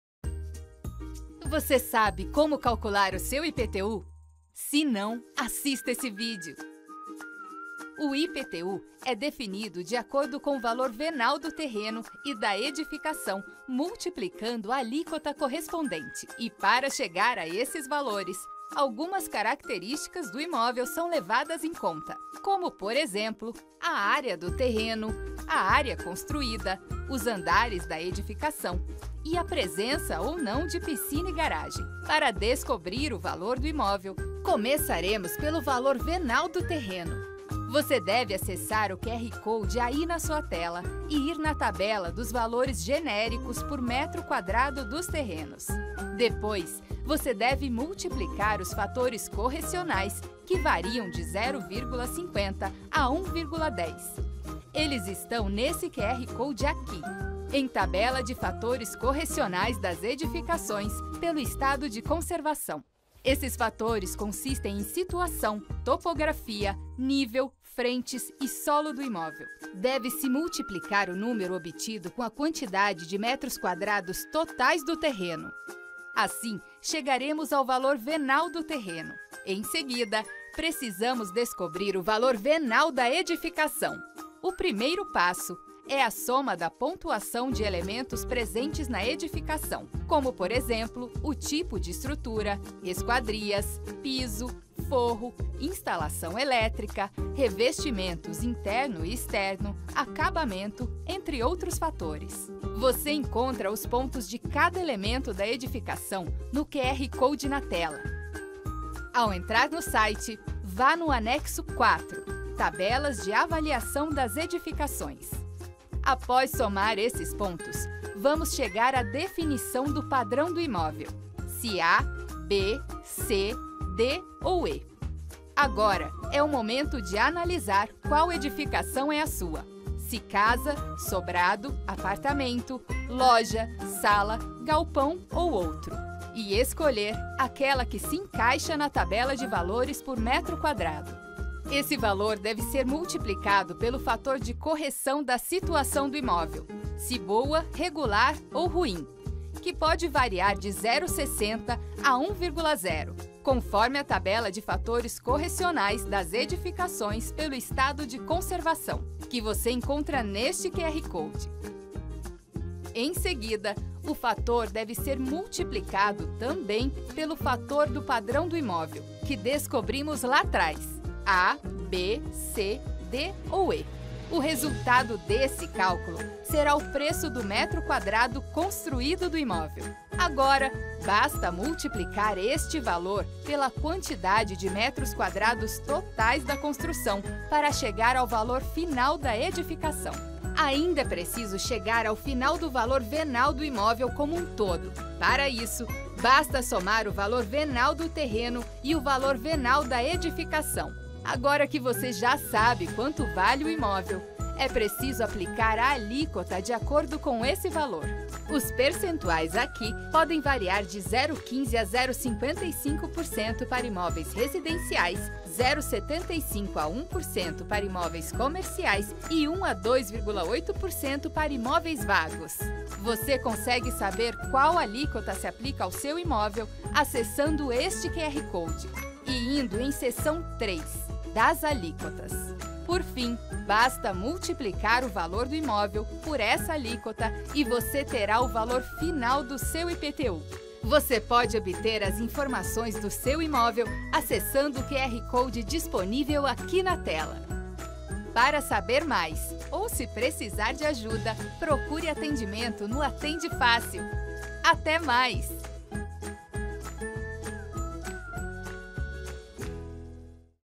IPTU 2024: Você sabe como calcular seu IPTU?
Confira o passo a passo e saiba todas as características relevantes no momento de calcular seu IPTU 2023.